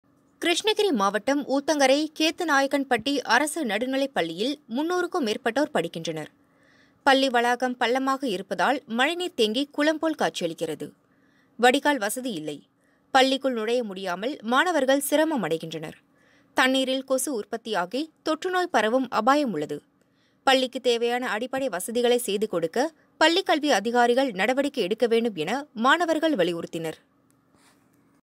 குளம் போல் மழைநீர் தேங்குவதால் நோய் பரவும் அபாயம்
கிருஷ்ணகிரி மாவட்டம் ஊத்தங்கரை கேத்துநாயக்கன்பட்டி அரசு நடுநிலை பள்ளியில் 300 க்கும் மேற்பட்டோர் படிக்கின்றனர்.

பள்ளி வளாகம் பள்ளமாக இருப்பதால் மழை நீர் தேங்கி குளம் போல் காட்சியளிக்கிறது. வடிகால் வசதி இல்லை. பள்ளிக்குள் நுழைய முடியாமல் மாணவர்கள் சிரமம் அடைகின்றனர்.